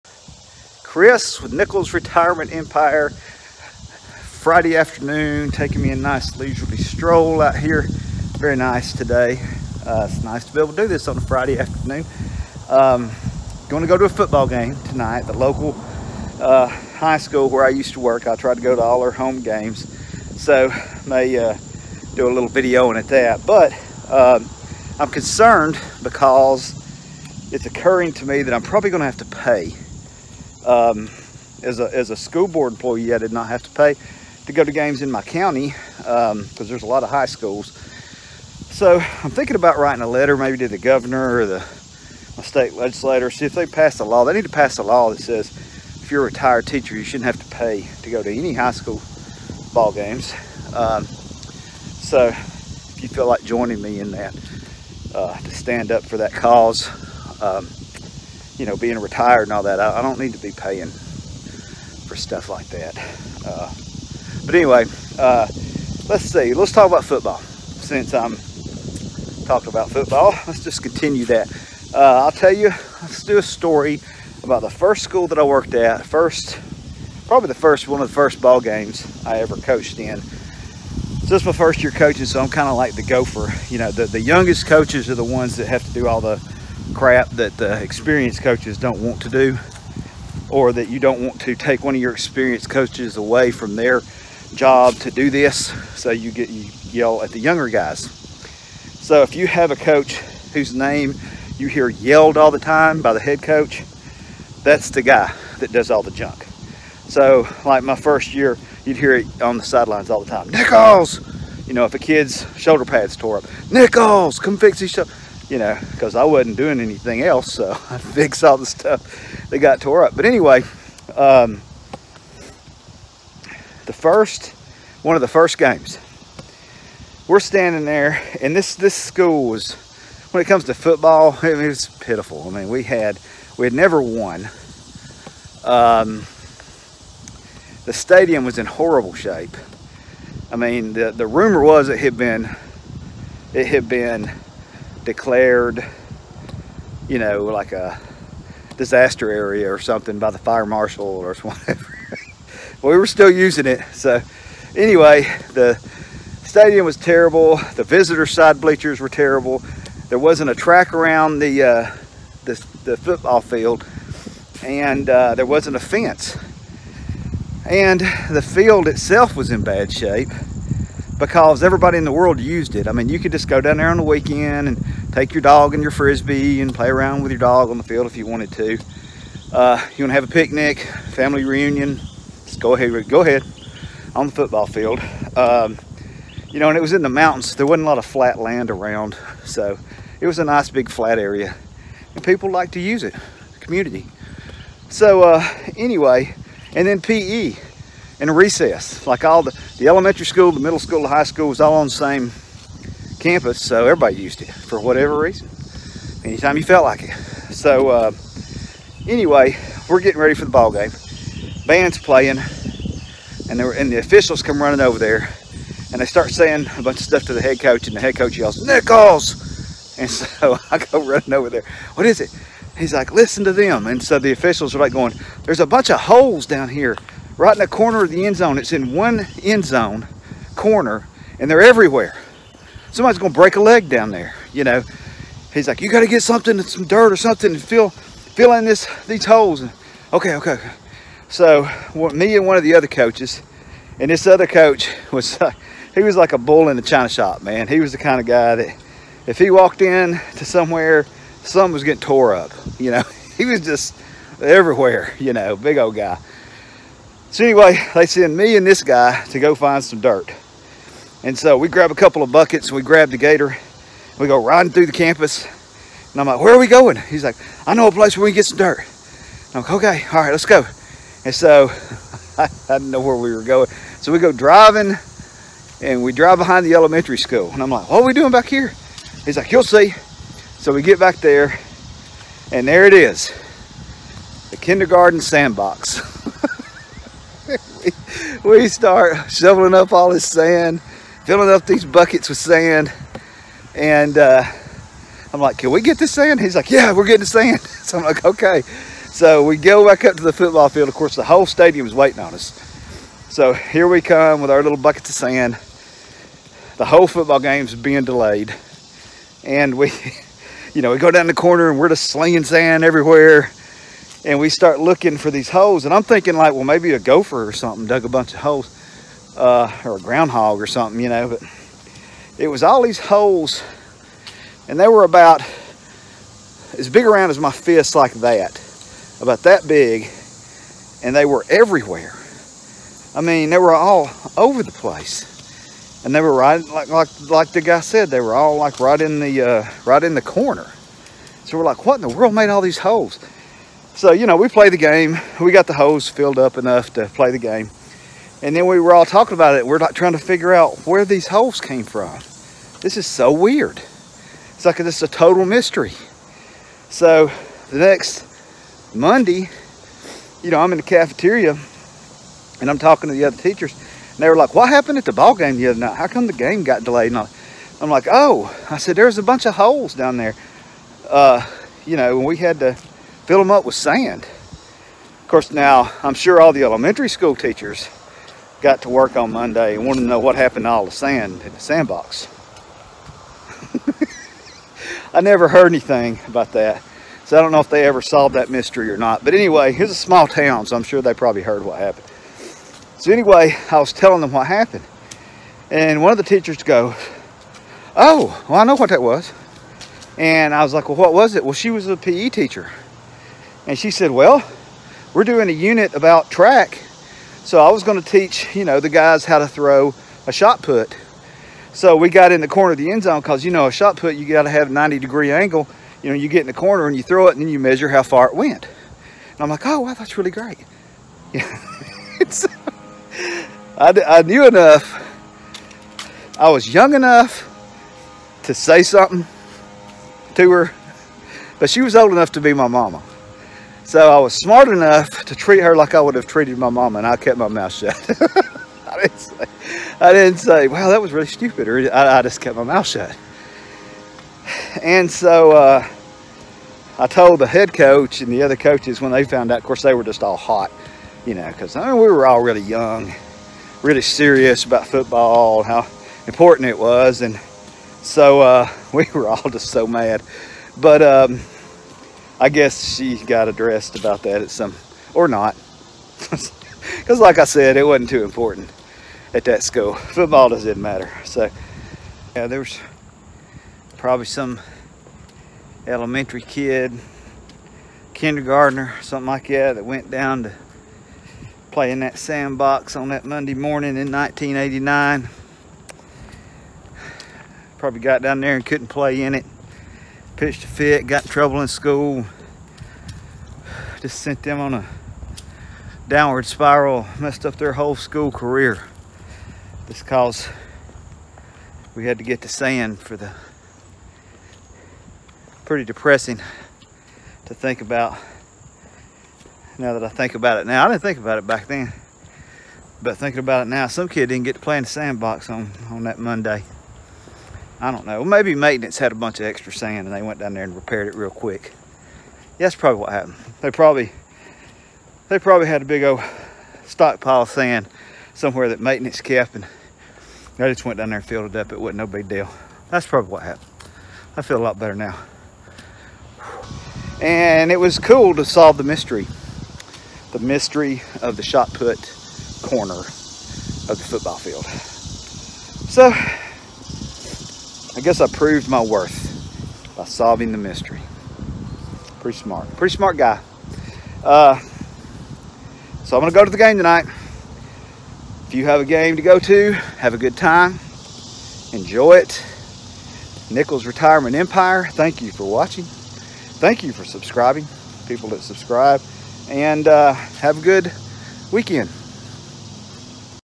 Holes-Nichols Retirement Empire
Holes-Nichols Retirement Empire. Here is a funny story about a mystery at one of the 1st football games I ever coached! Many of you may be going to high school football games this week so I thought this would be a good time to tell about it!